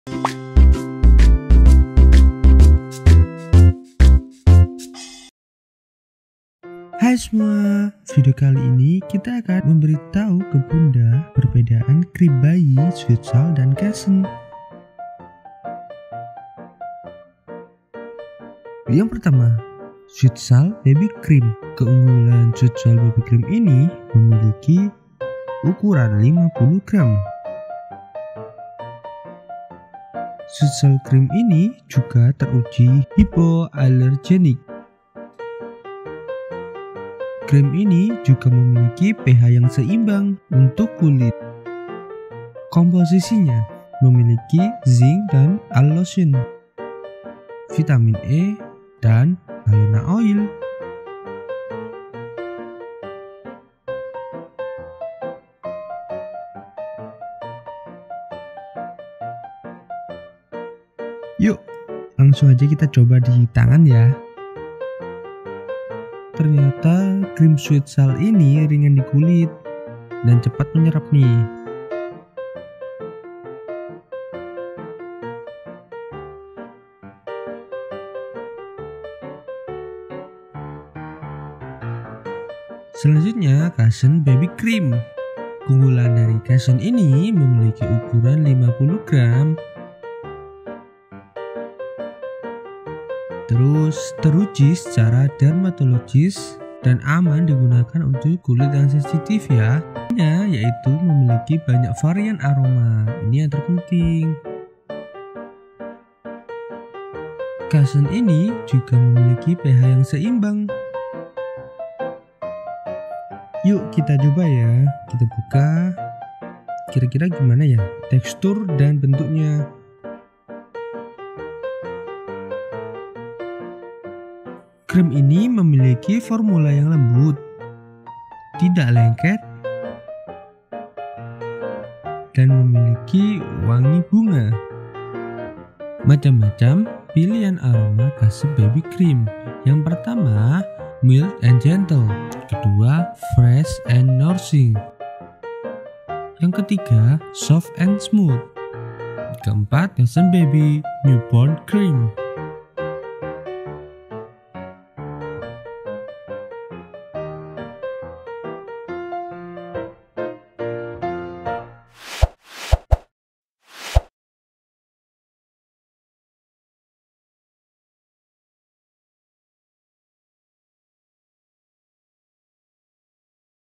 Perbedaan Cream Bayi Cussons & Zwitsal
Cream Baby sangat membantu untuk menjaga kelembapan kulit bayi. dan video kali ini kita akan mengupas perbedaan cream bayi dari zwitsal dan cussons. Kira-kira apa aja ya perbedaan dari kedua cream ini? apakah semua nya rekomen untuk kulit bayi.

*** MARKETPLACE ***

*** SHOP ADDRESS ***

jl. kalibokor no 81 surabaya
(belakang puskesmas pucang)